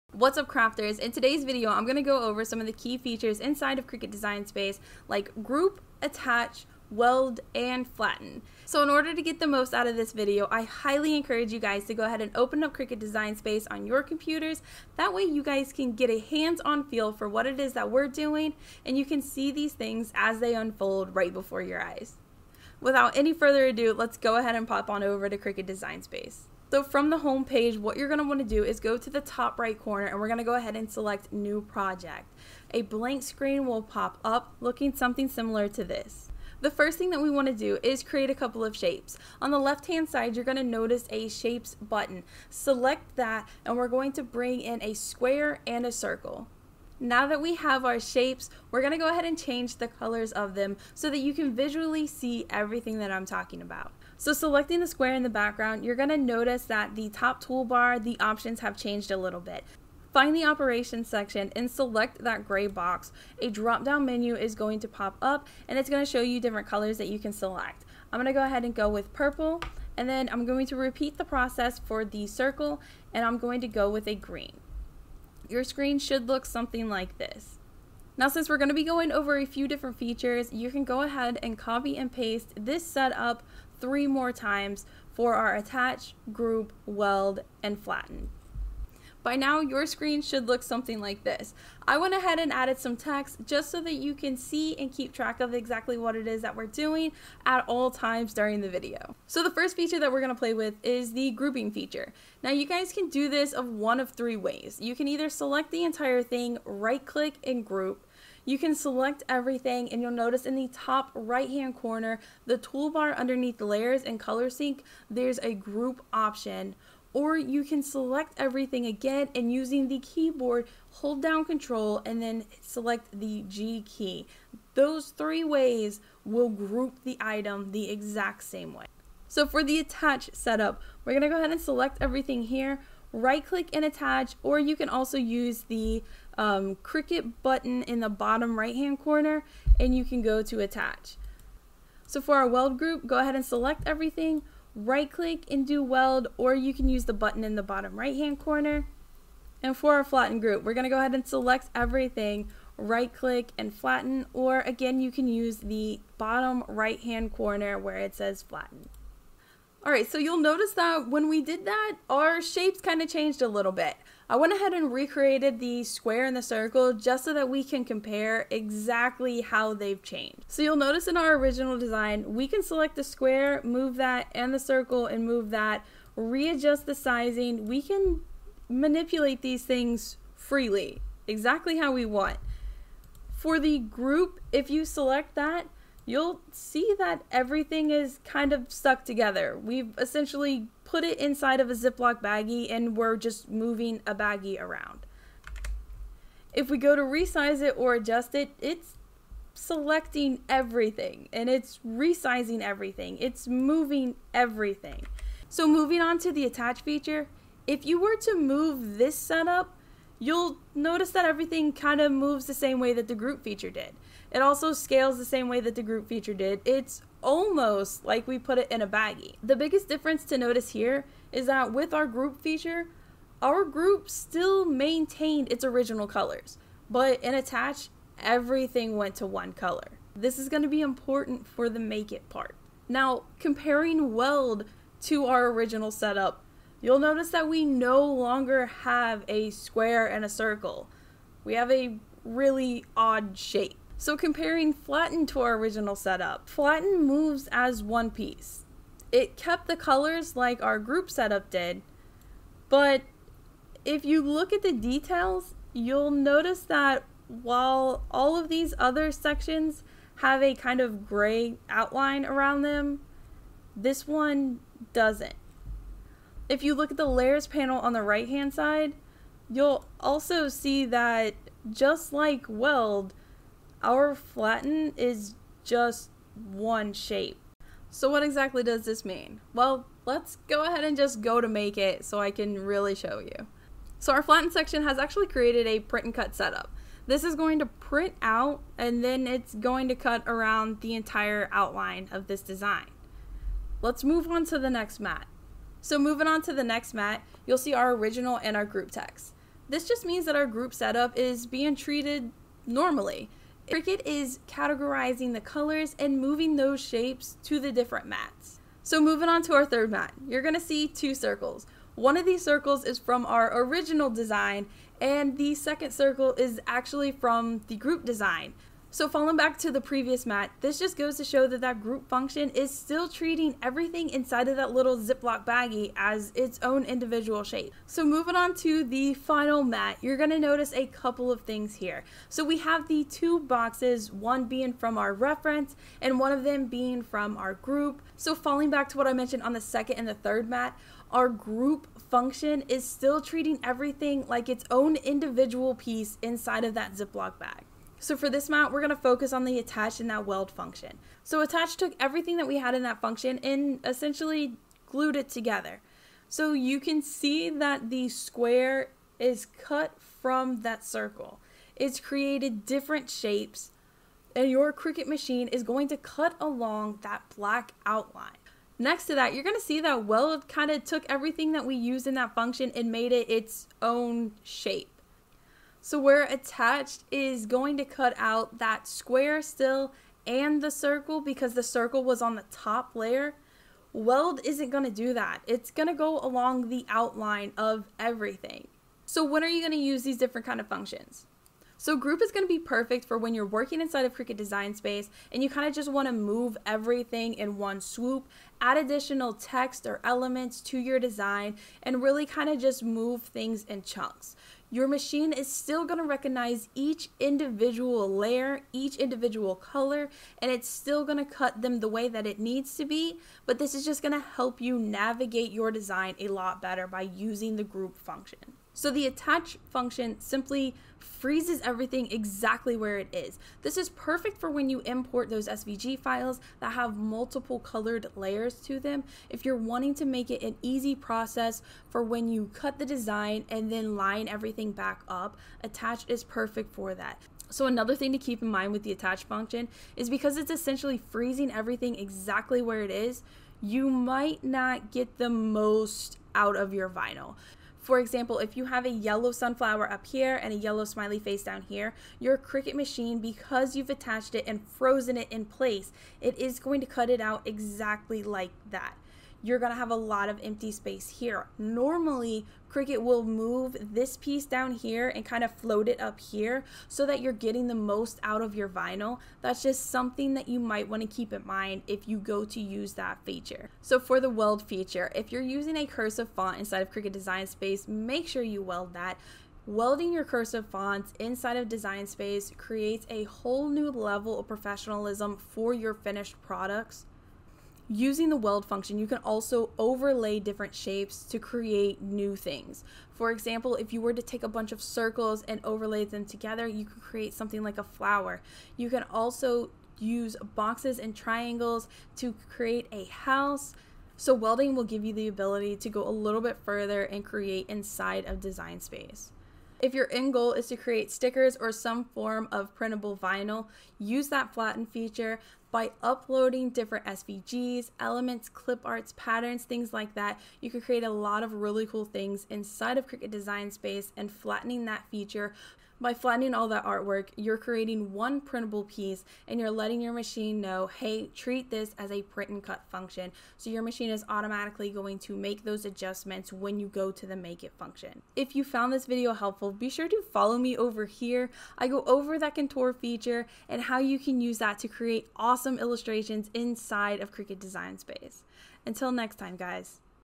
What's the difference between Group, Attach, Weld, and Flatten in Cricut Design Space
Cricut design space doesn't have to be hard! If you want to know more about the Cricut functions Group, Attach, Weld, and Flatten, take a minute to look through this video. I highly encourage you to open the design space yourself so you can follow along.

Need a more in-depth look at Design Space Check out Design Space 101 tutorials!

The Socials